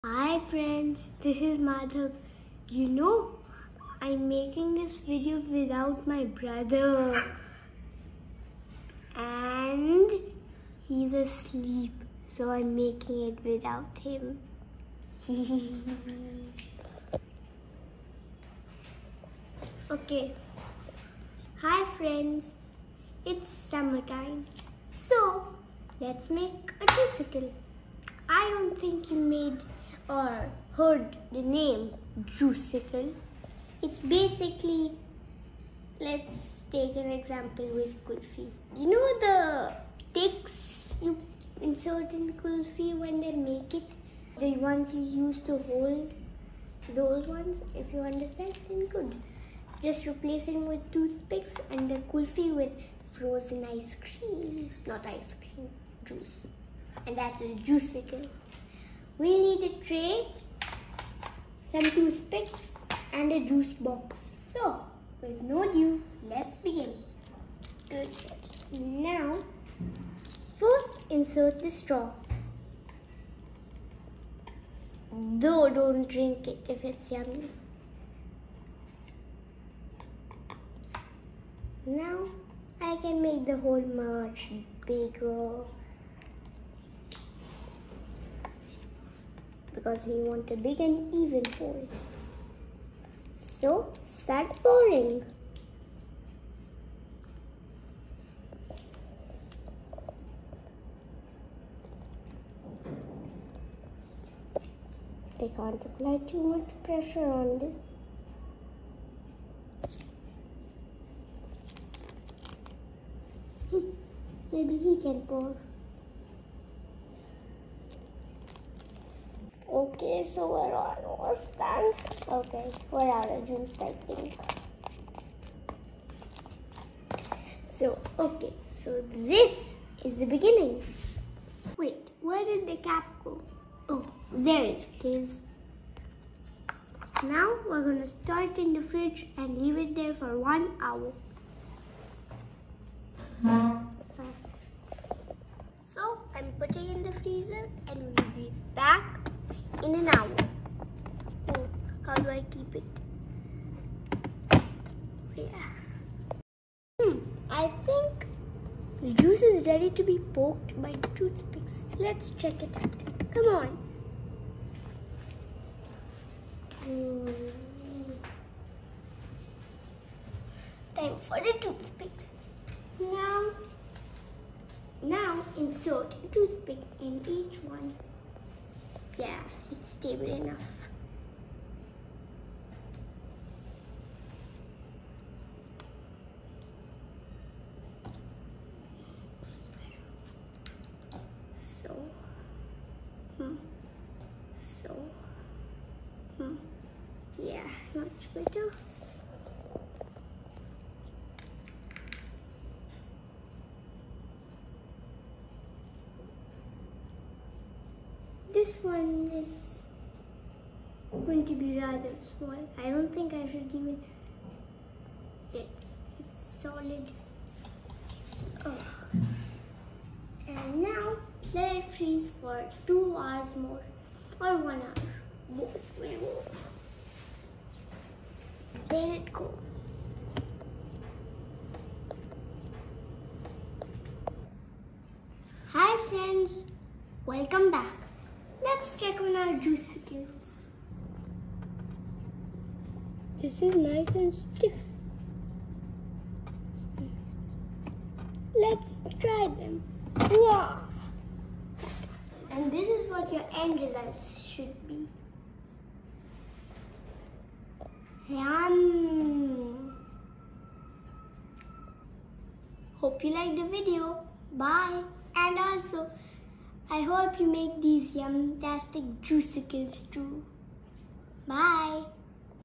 How to make
Summers are here..and we all enjoy juices.. cold drinks.. and ice-creams.. this brings in an idea in Madhav to make Juicicles... which he now enjoys everyday...

Make Juicicles with him and have fun..